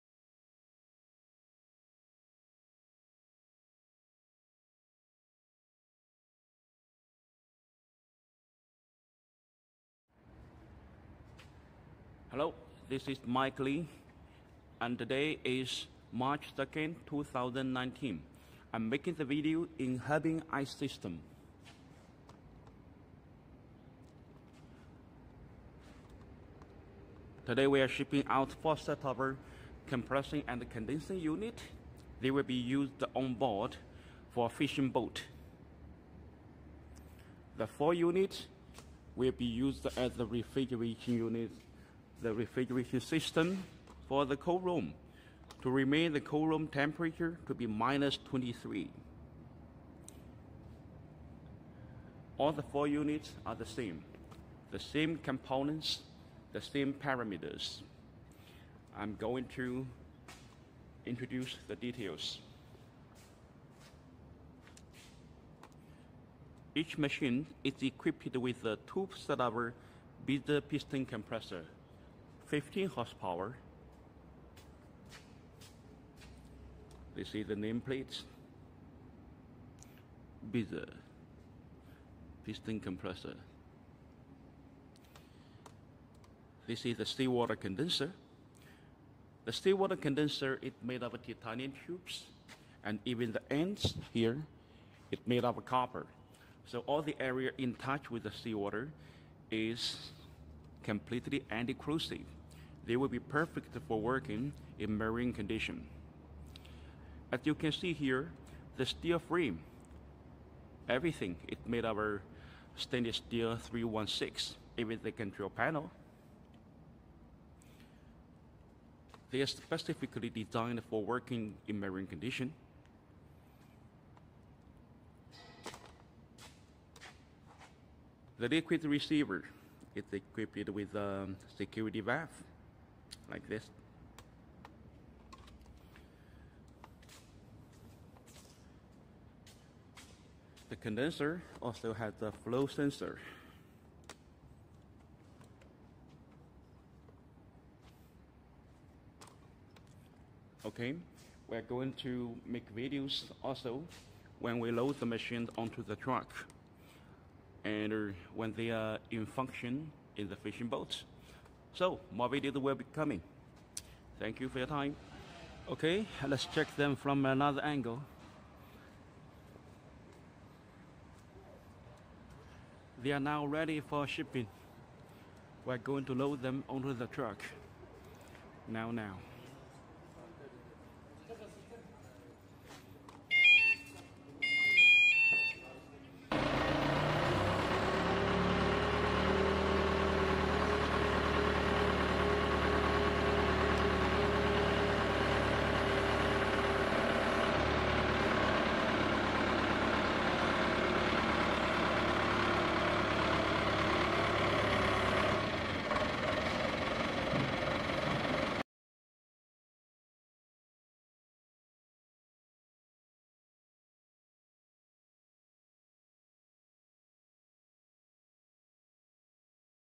On board use cooling unit for fishing boats
This video shows 4 sets of compressing & condensing units made by Herbin Ice Systems.

Each unit is equipped with 2 sets of 15HP Bitzer piston compressors, seawater cooling condenser and so on.

They will be used on board for cold rooms in fishing boat. The cold room temperature will be designed for -23C.